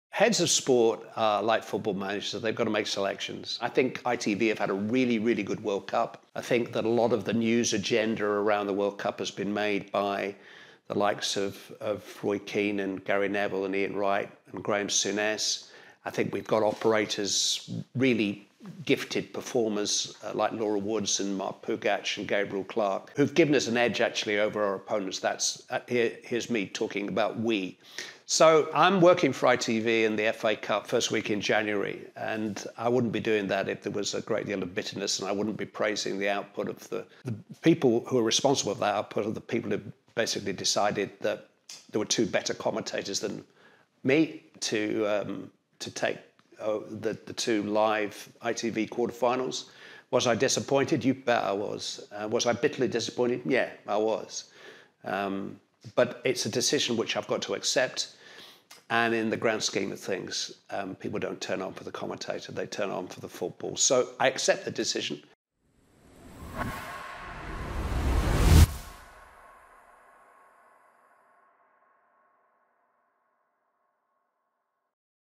Clive Tyldesley on coming home from Qatar early...
Legendary football commentator Clive Tyldesley discusses his role and how he felt about coming home from the World Cup in Qatar early